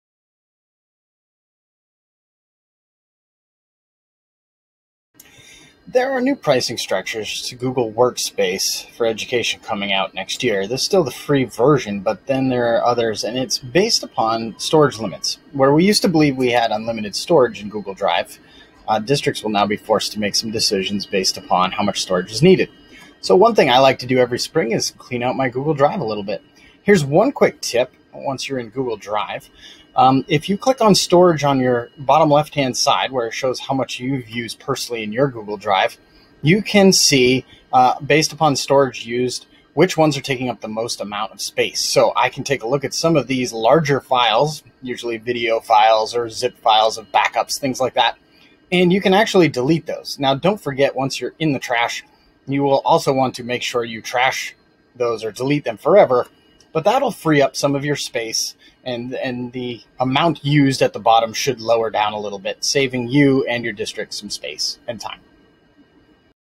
Finding and Deleting Large Files in Google Drive
In this short video tutorial, I demonstrate how you can easily identify your largest files in Google Drive. This way you can back up your large files or delete them to free up Google Drive space.